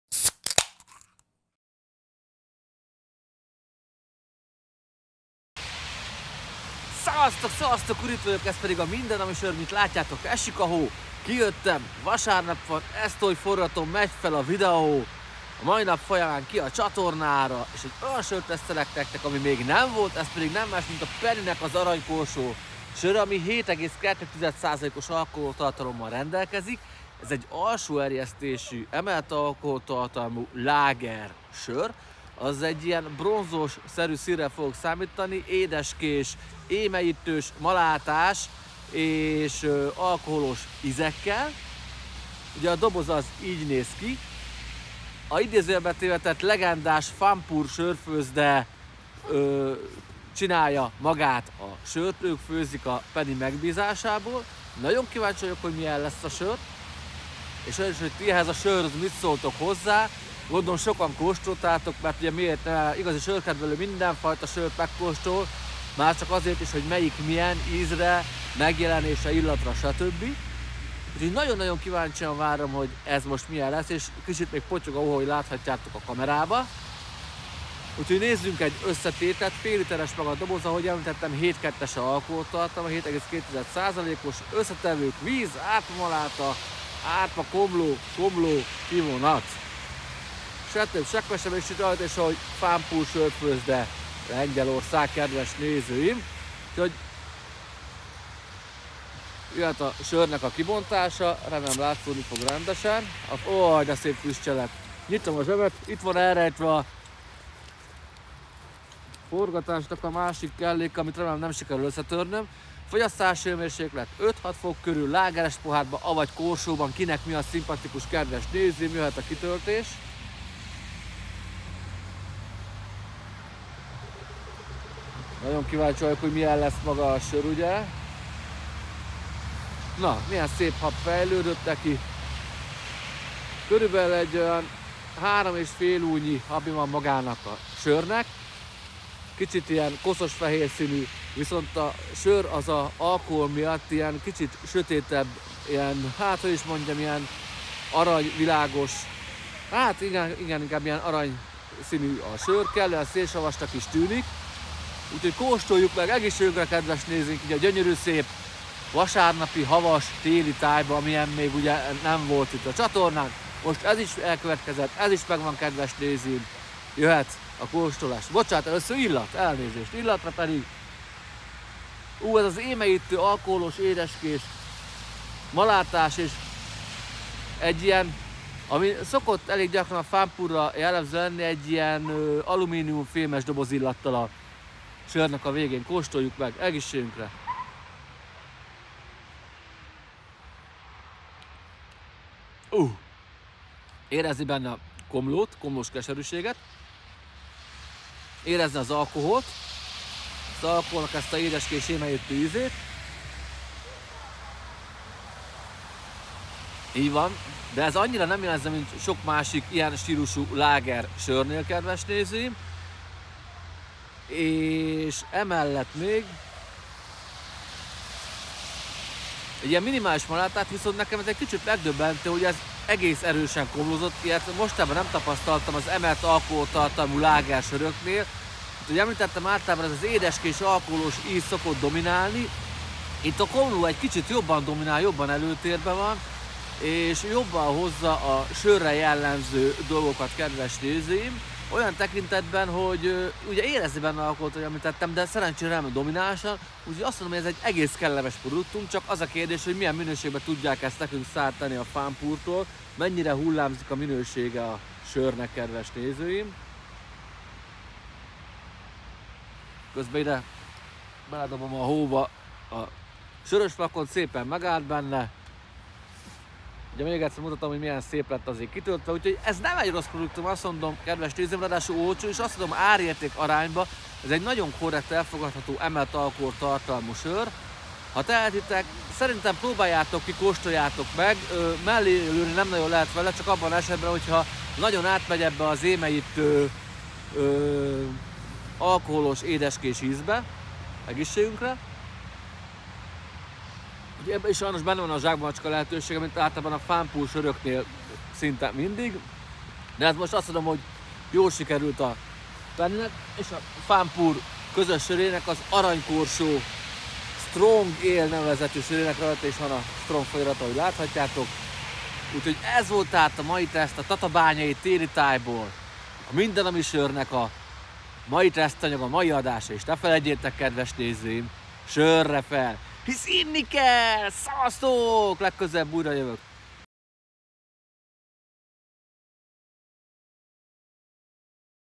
Arany Korsó Strong Lager Lengyel Sör Teszt
Ha szeretnél teszt anyagot küldeni keress bátran. Köszönöm

Milyen a Penny alkohollal szétcsapott söre???
Émelyítő, vagy azért jobb mint azátlag strong lager sör??
A tesztből kiderül.

A háttér szándékos :)
Első havas téli sör tesztem :)
A gyerekek mellettem voltak azért volt a sok kitekintés oldalra...
Próbálták apát megdobni :D